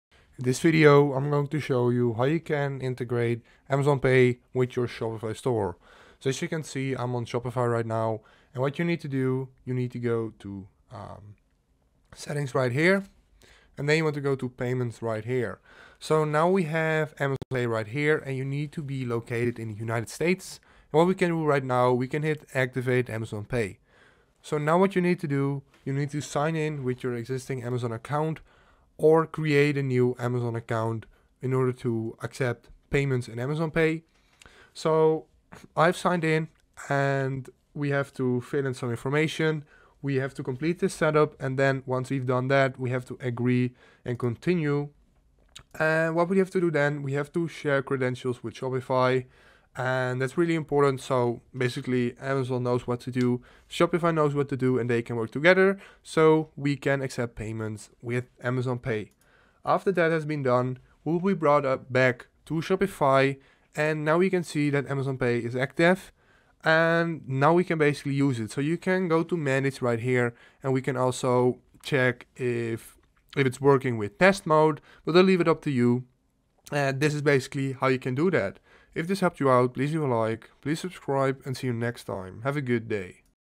How to Add Amazon Pay to Shopify (Quick & Easy)
Learn How to Add Amazon Pay to Shopify (Quick & Easy)

In this video I show you how you can add amazon pay to shopify. Do you wonder how you can connect amazon pay to shopify. I'll show you exactly how to do that.